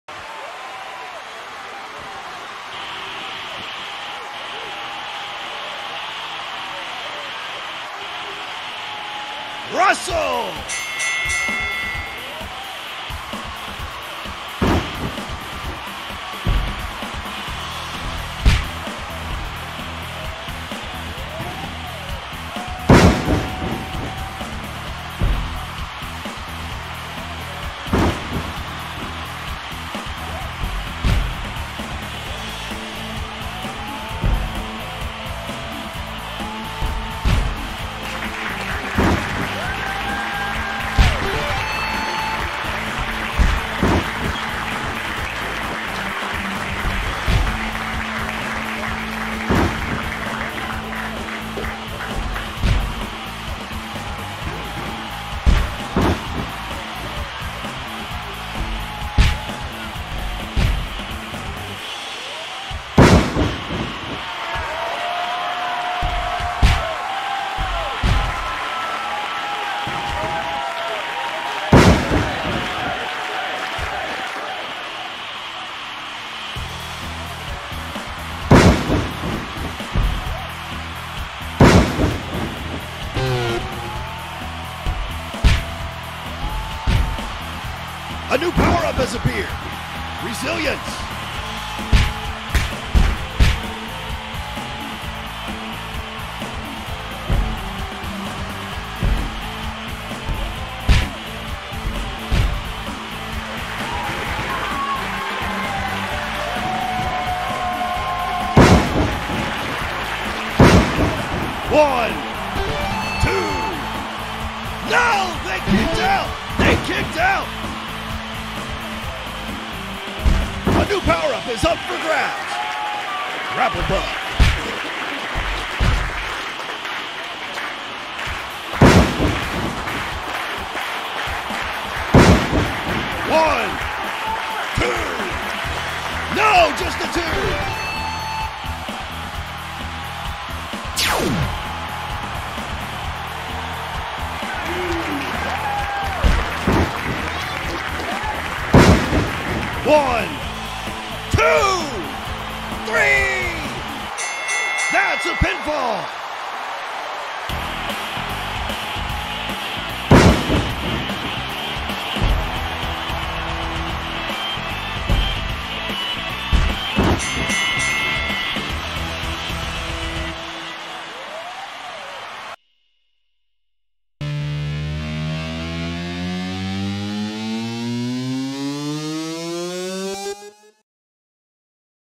Mr. I Vs Dry Bones
It's the Battle of the Scary Monsters as Mr. I, the Mobile Eyeball of Muscle, take on Dry Bones, the Undead Koopa.

Mr. I and Dry Bones go to Nintendo
Both wrestlers are modeled by @kusanagi_wrestler